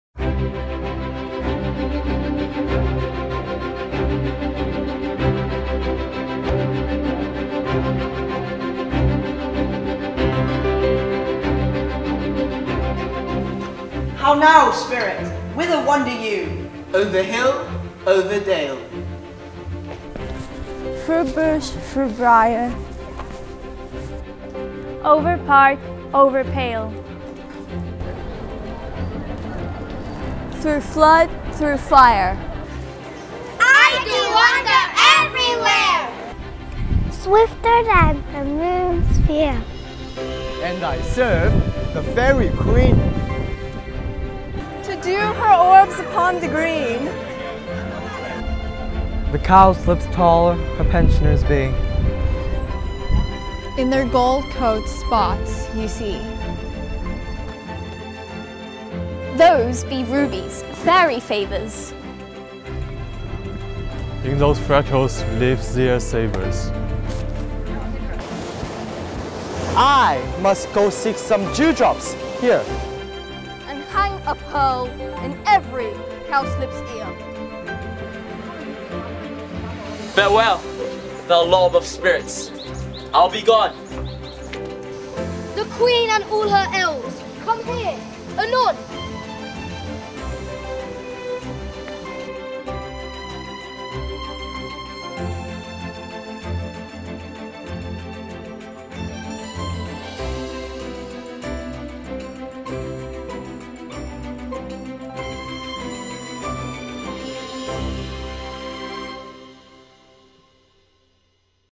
Dulwich International - Shakespeare Film
Celebrating 400 Years of Shakespeare, our amazing Dulwich students showcase their international network around the globe with this video, ‘All the World’s A Stage’.